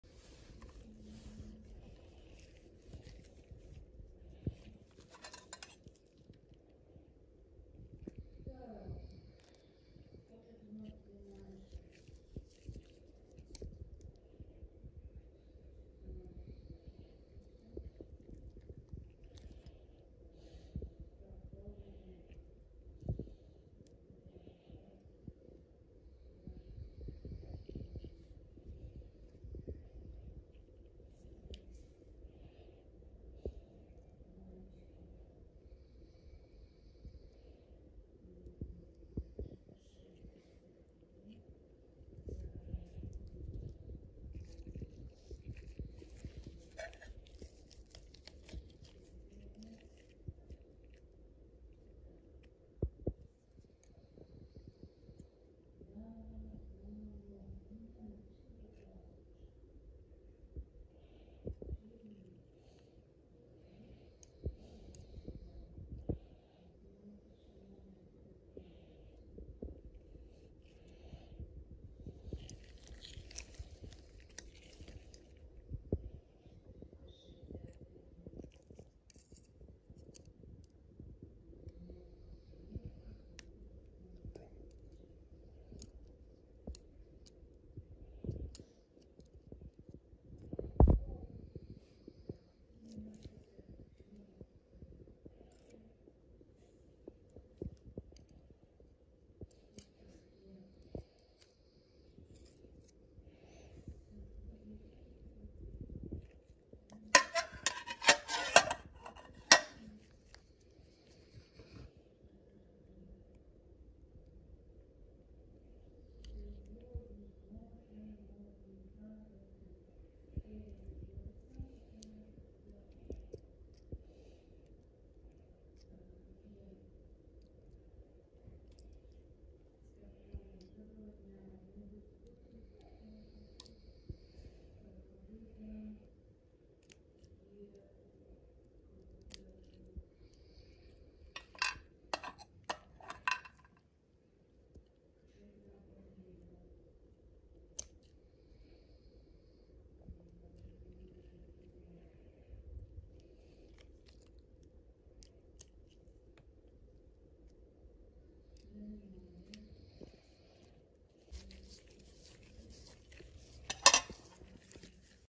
Removing ticks from the Belly after a walk in great smoky mountains
The great smoky mountains are a great place for hiking, but you need to be on the alert and wear protective clothing so that ticks do not attack you. In this video you will see unique footage of the removal of ticks and parasites.